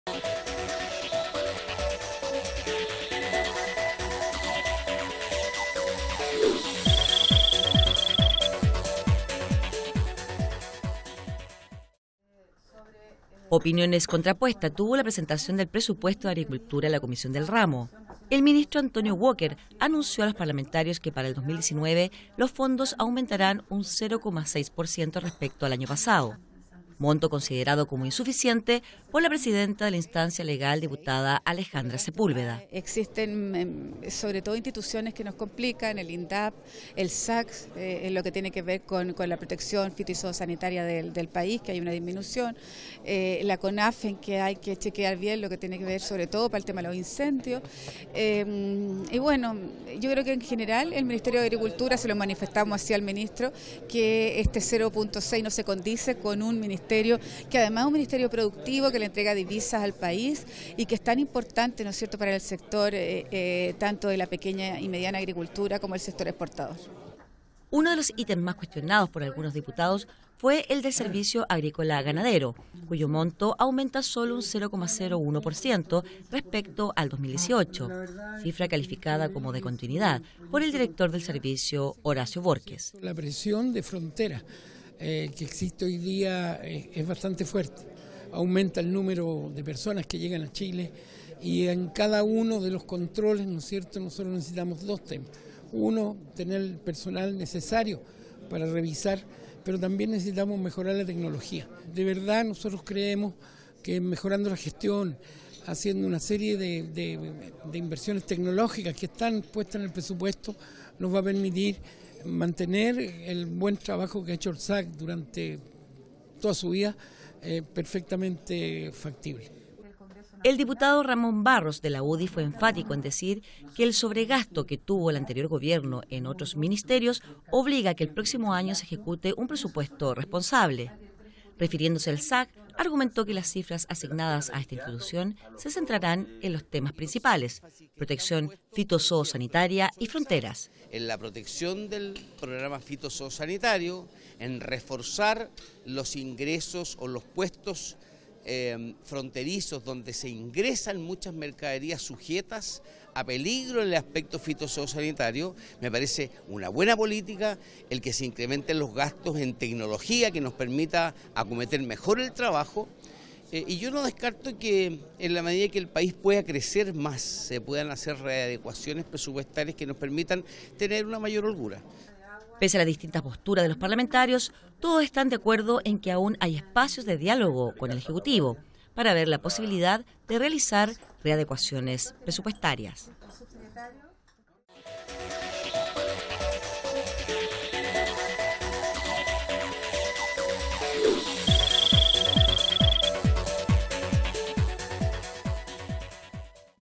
Discrepancias genera presupuesto 2019 de Agricultura
Un intenso debate tuvo como escenario la comisión de Agricultura que, para conocer el presupuesto 2019 de dicha cartera, recibió al ministro Antonio Walker junto a los titulares de organismos como el Instituto de Desarrollo Agropecuario, Conaf, Odepa, la Comisión Nacional de Riego, el INIA y el SAG.